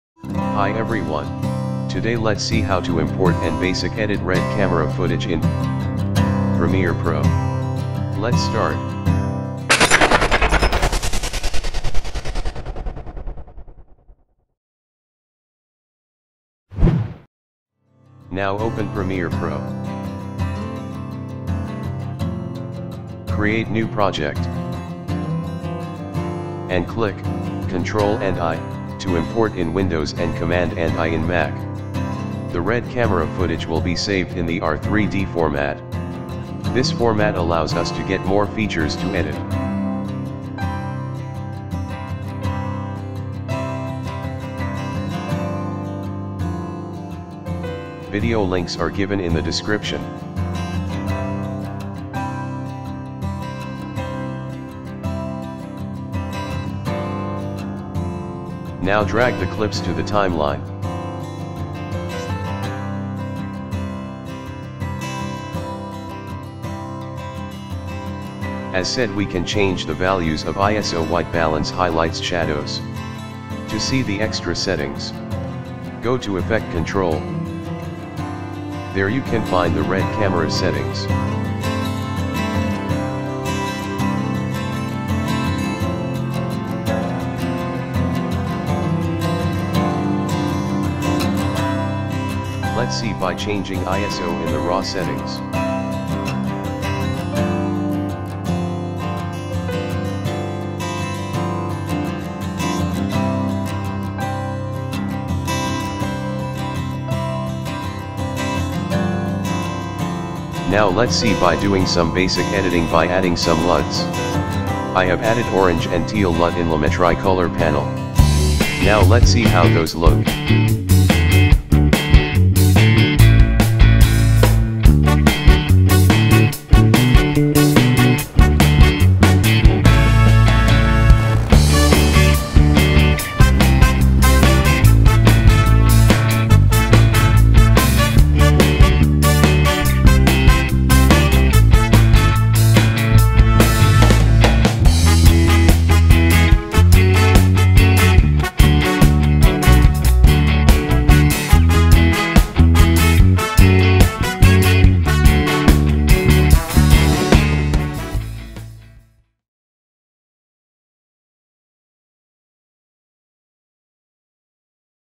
How to import and edit RED raw footage in Premiere Pro || REDITING
This tutorial shows you the best way to import RED footage (.rd3) into premiere when you have extended clips. Extended clips are clips that are segmented when shot due to their length. Normally they appear in folders among the regular clips. This TUTS shows you how to import them as straight clips.